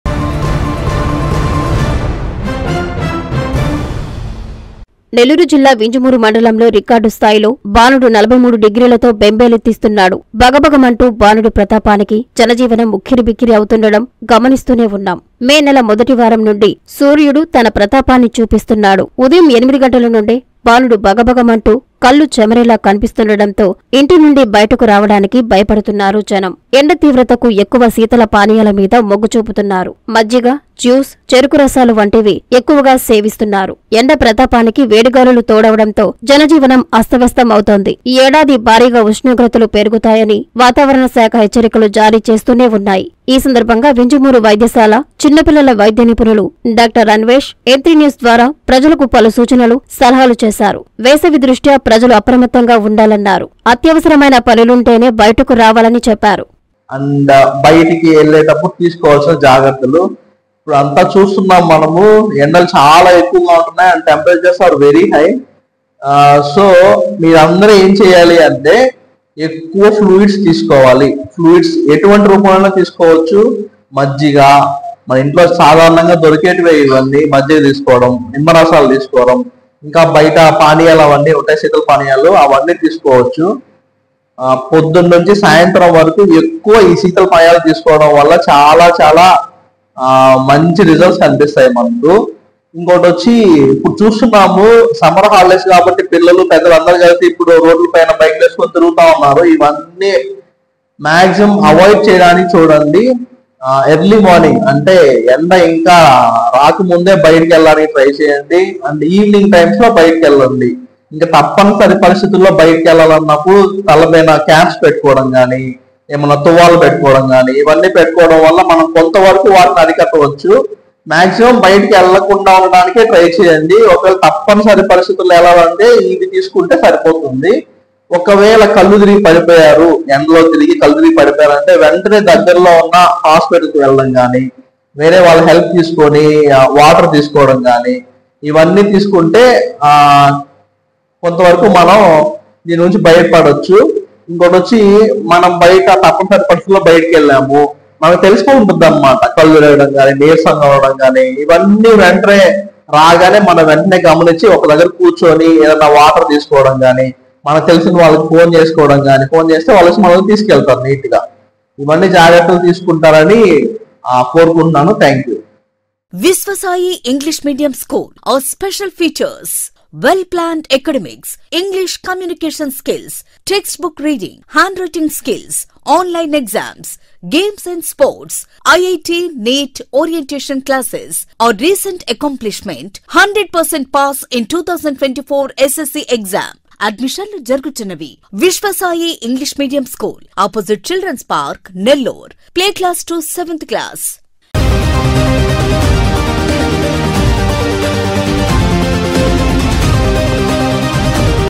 భ‌గ్గుమంటున్న భానుడు
- వింజ‌మూరులో @43 డిగ్రీలు
- అల్లాడిపోతున్న జ‌నం
- ప‌లు సూచ‌న‌లిచ్చిన చిన్న‌పిల్ల‌ల వైద్య‌నిఫుణులు డాక్ట‌ర్ అన్వేష్‌